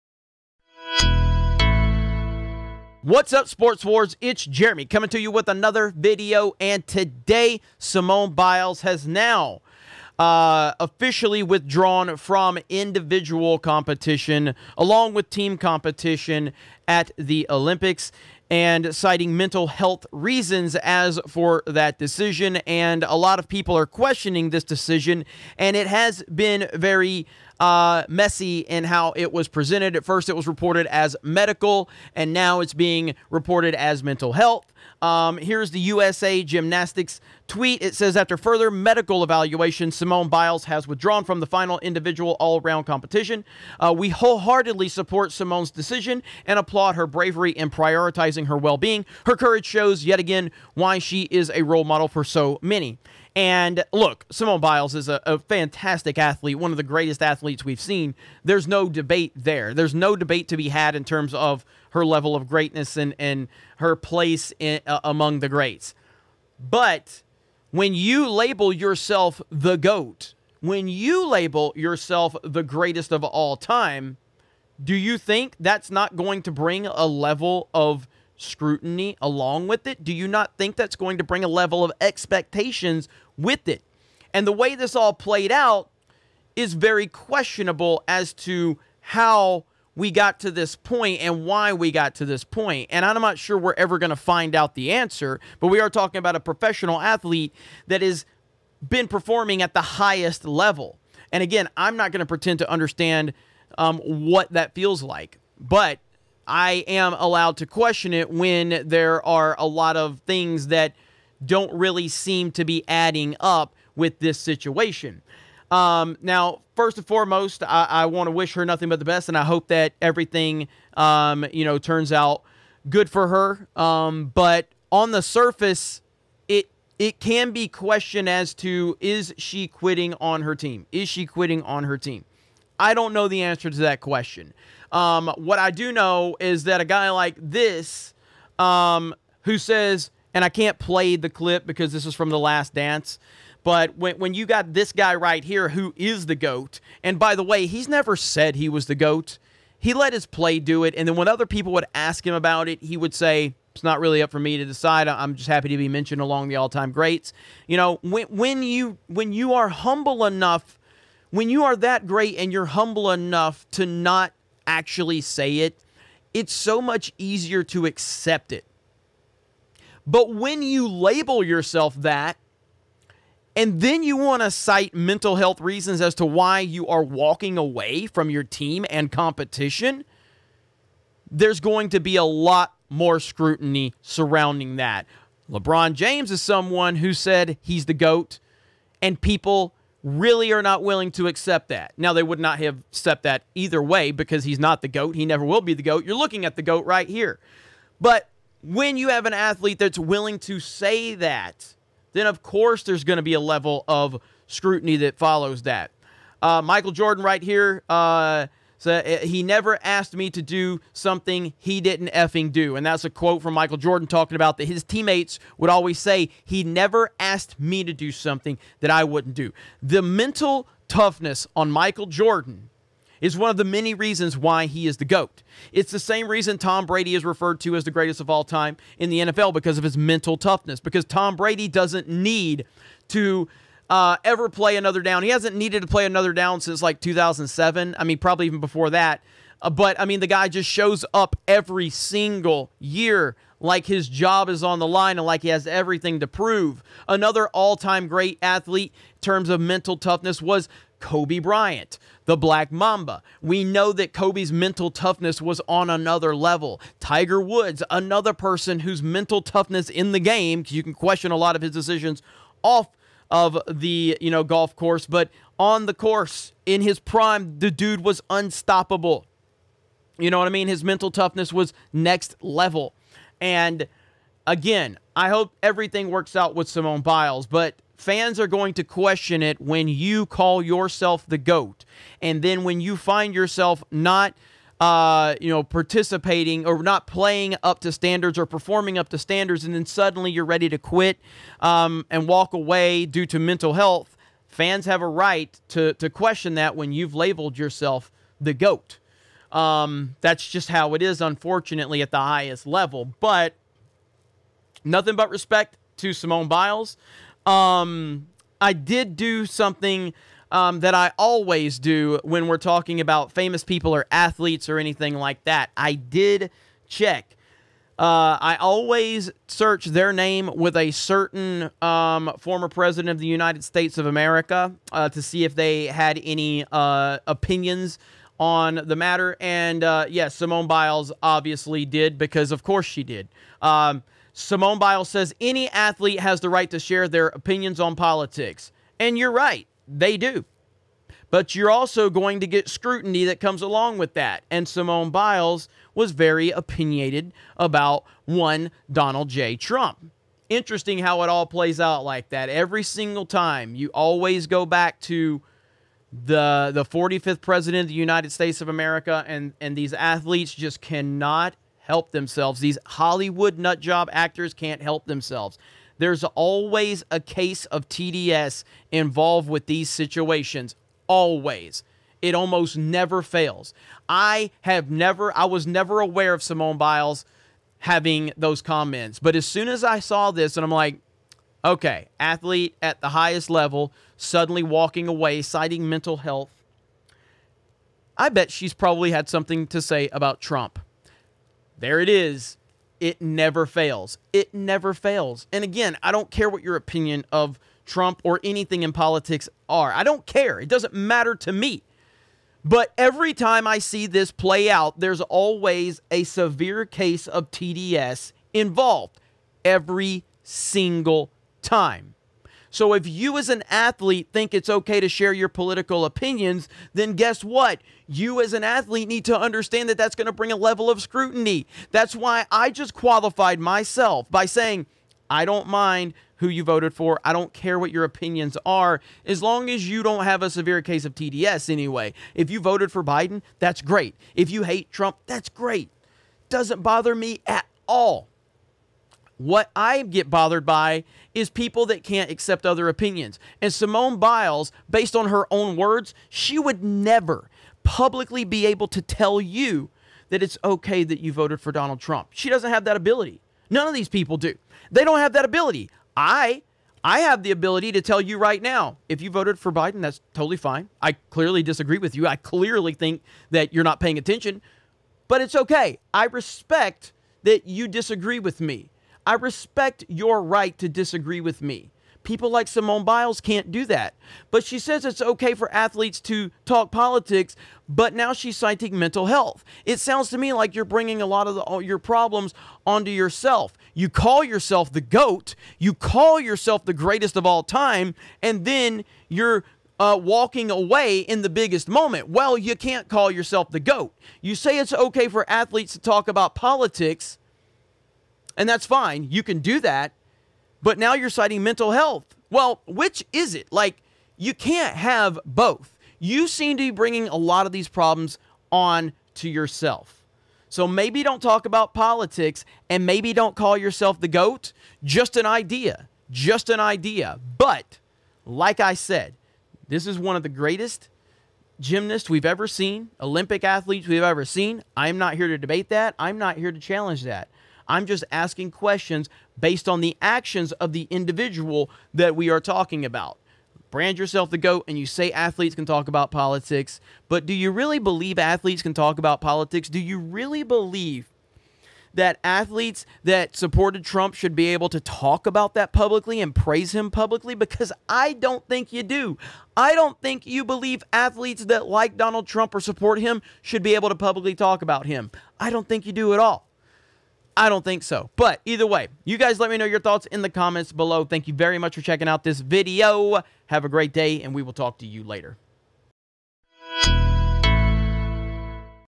Simone Biles WITHDRAWS From Individual All-Around Final Over "Mental Health" - Tokyo Olympics
Jeremy Prime
Pensacola
Florida, USA
32524